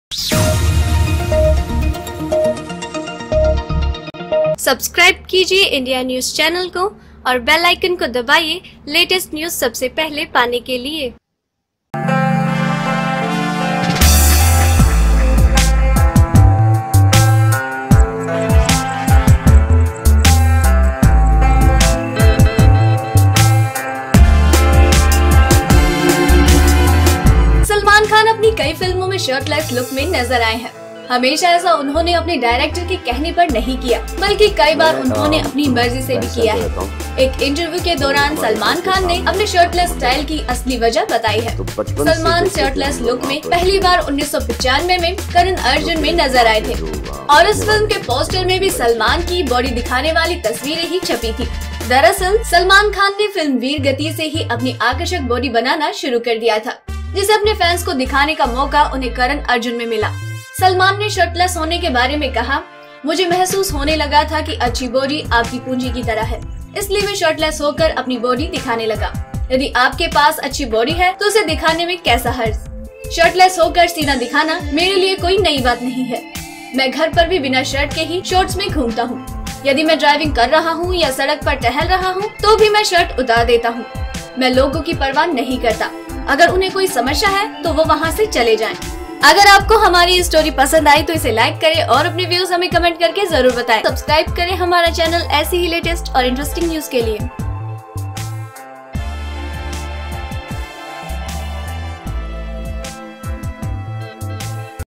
Salman ने सबके सामने उतारे अपने कपडे !!
Salman Khan has appeared in shortsless look in many films. He always did so on his own request, but not on the request of his director, but on many occasions himself. During an interview, Salman told the real cause of his shirtless style.